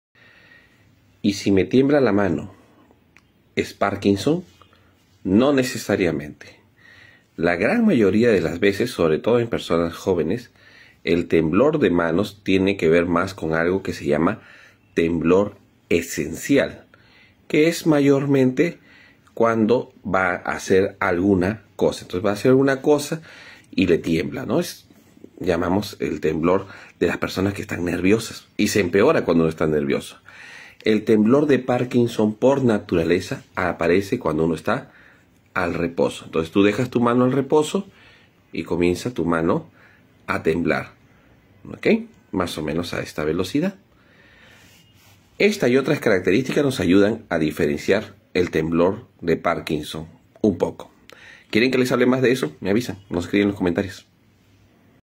¿Los temblores en las manos significan que tengo Parkinson?
El Dr. Carlos Revoredo diferencia entre el temblor de Parkinson y el temblor esencial.

En Meddi cuidamos la salud del Adulto Mayor y su familia para un envejecimiento saludable.